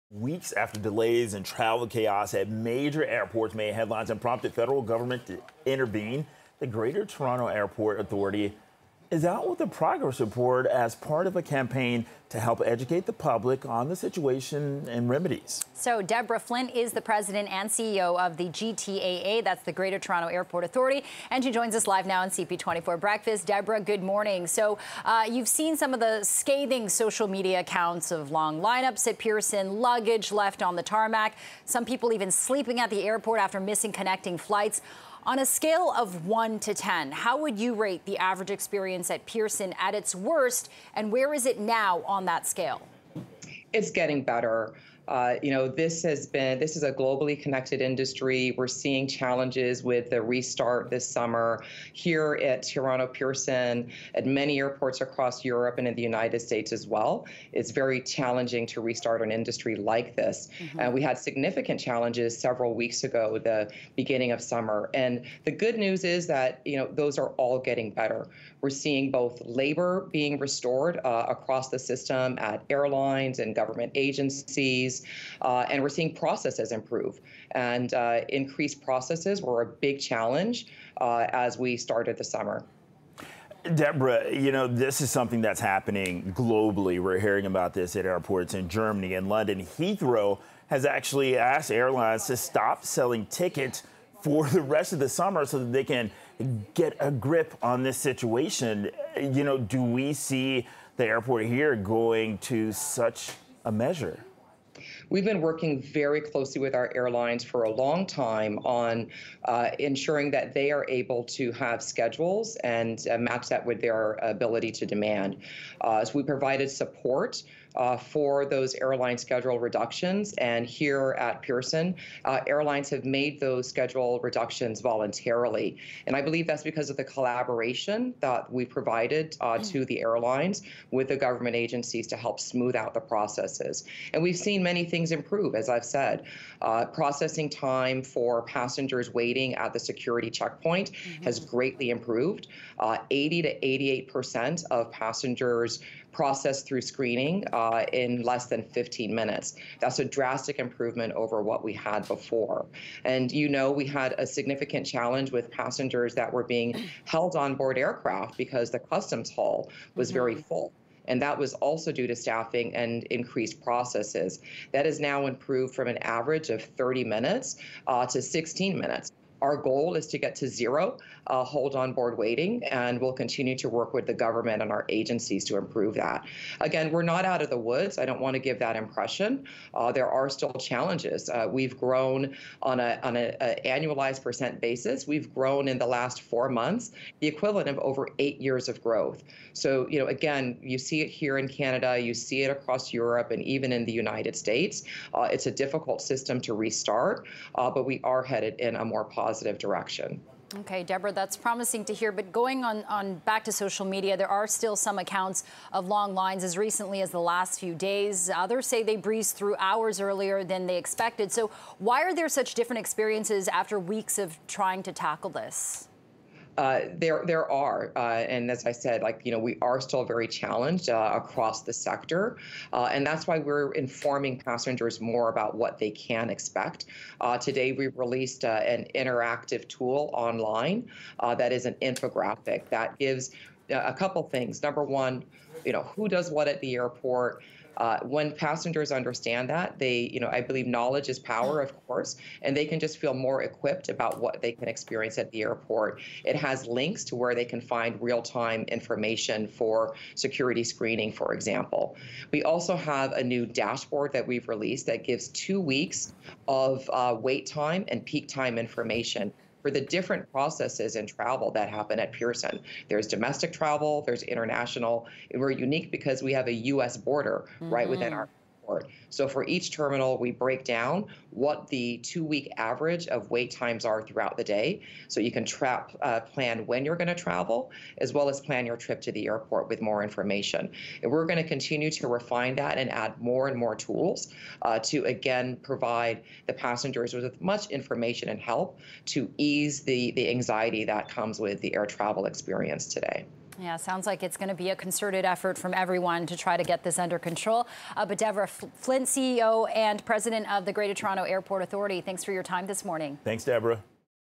GTAA says progress being made at Pearson airport
Weeks after delays and travel chaos at major airports made headlines and prompted federal government intervention, the Greater Toronto Airport Authority is out with a progress report as part of a campaign to help educate the public on the situation and remedies.